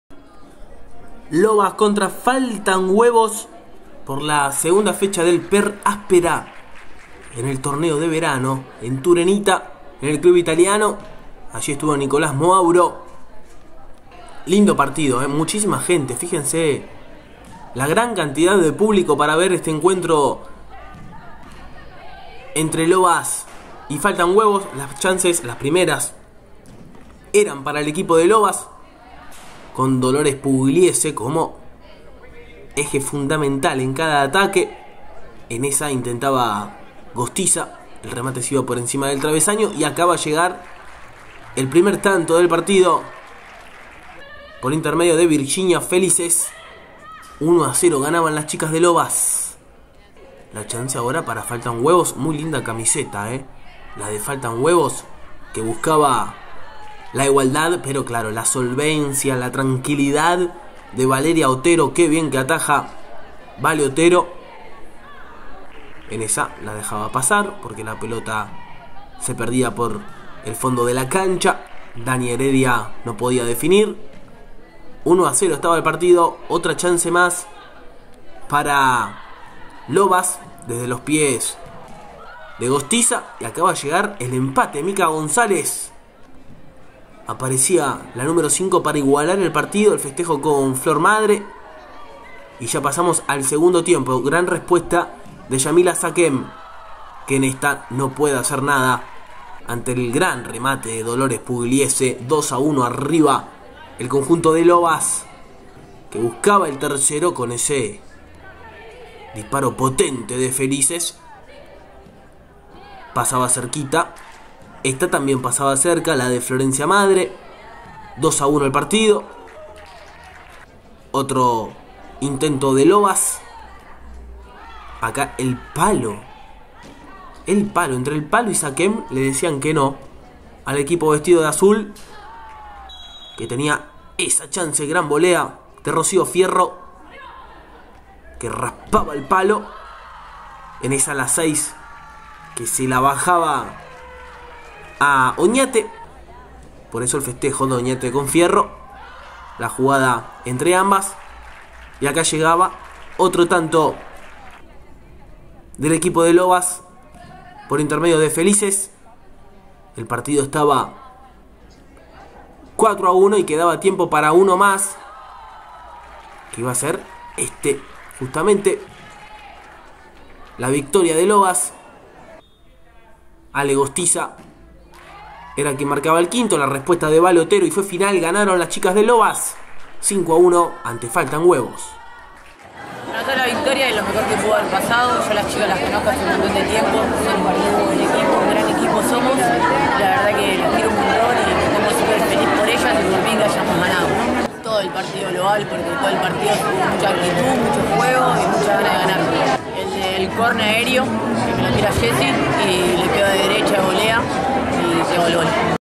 Torneo de Verano: Lobas - Falta Huevo
Lobas arrancó mejor en el partido, y luego de varias chances se puso arriba en el marcador, pero Falta Huevo reaccionó a partir del gol y también tuvo sus posibilidades y las aprovechó: Micaela González marcó el empate. Pero en el segundo, las de azul sacaron diferencia y finalmente lo ganaron por 5 a 1. "En todo el partido hubo mucha actitud, mucho juego y ganas de ganar", expresó la figura Virginia Felice, quien marcó su doblete.